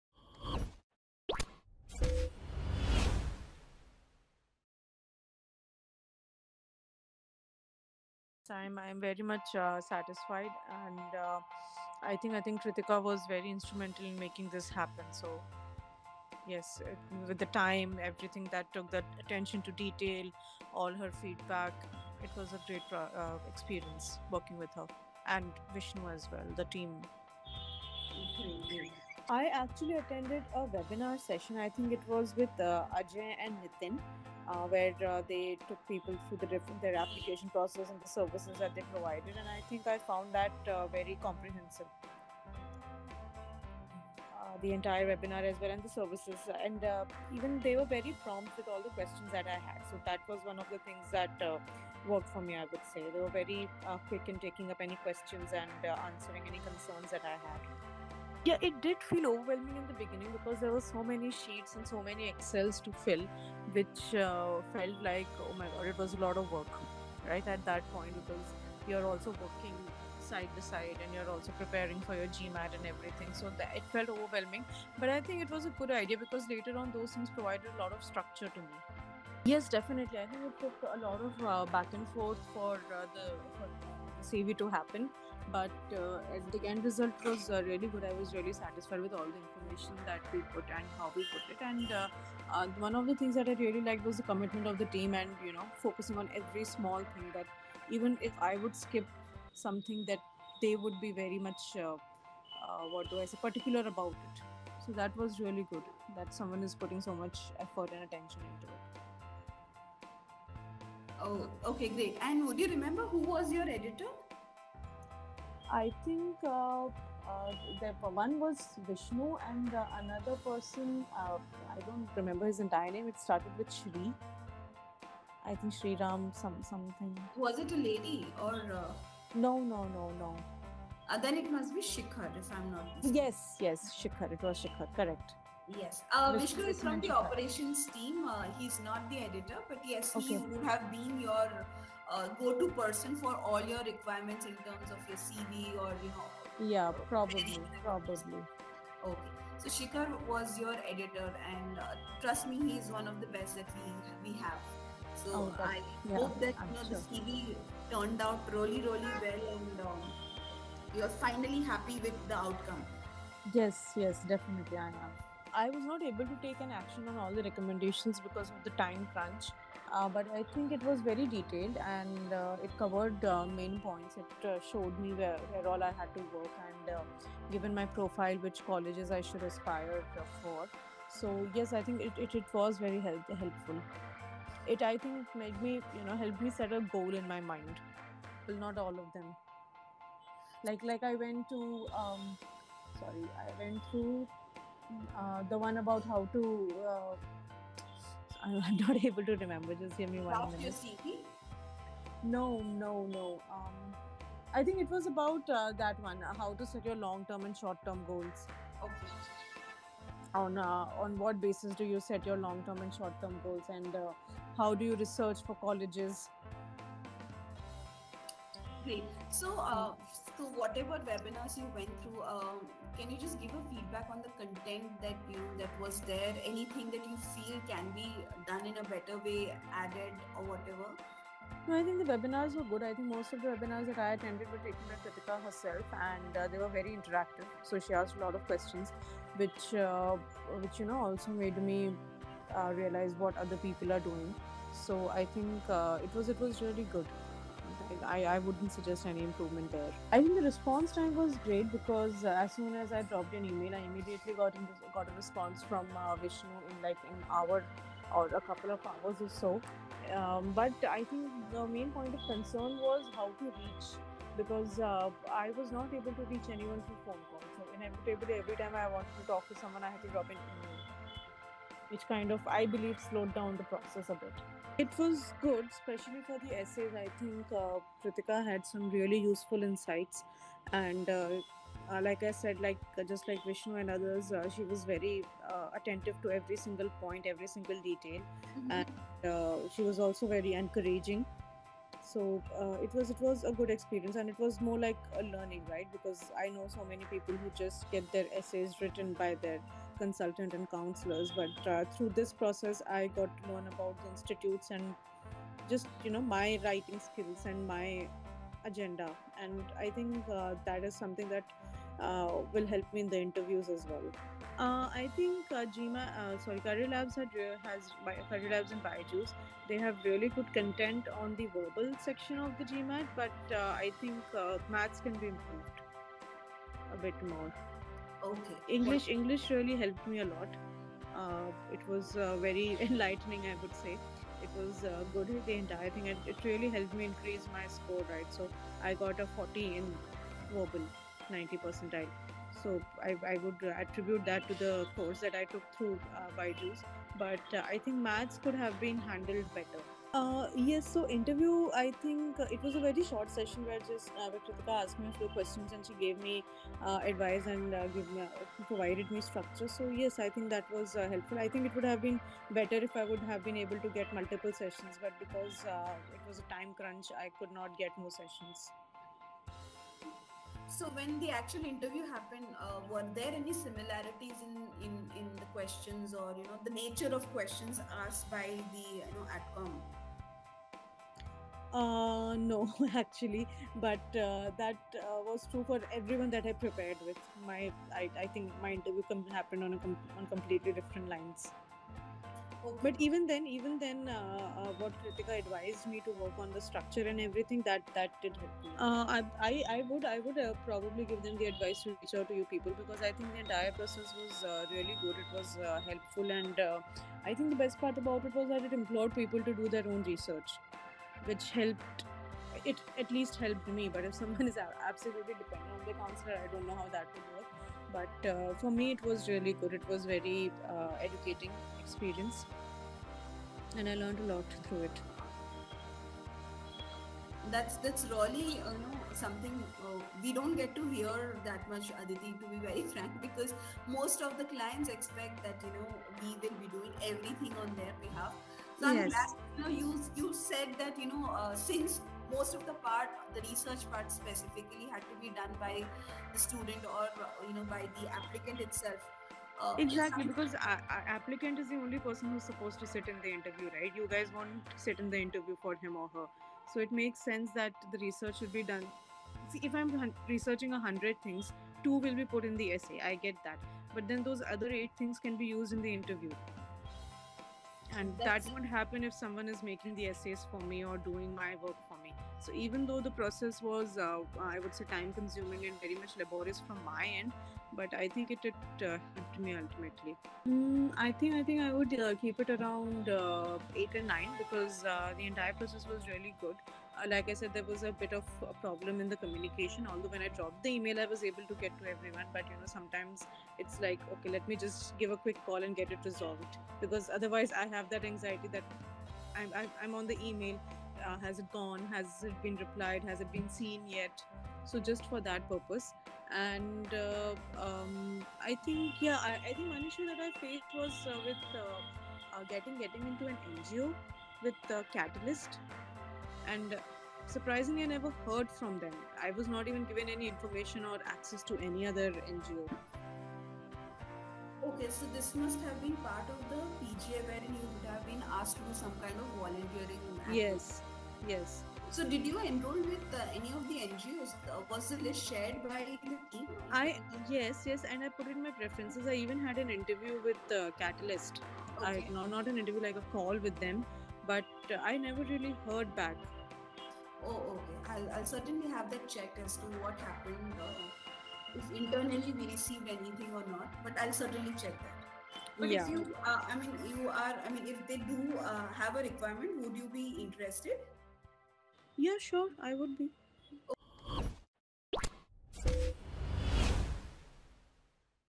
Hear From Our GMAT Top-Scorer - Aditi | GMAT Preparation Program (Personalised), By CareerLabs
and grab your chance to learn from 780 Scorer & ISB Alumnus - Santosh Sir, today!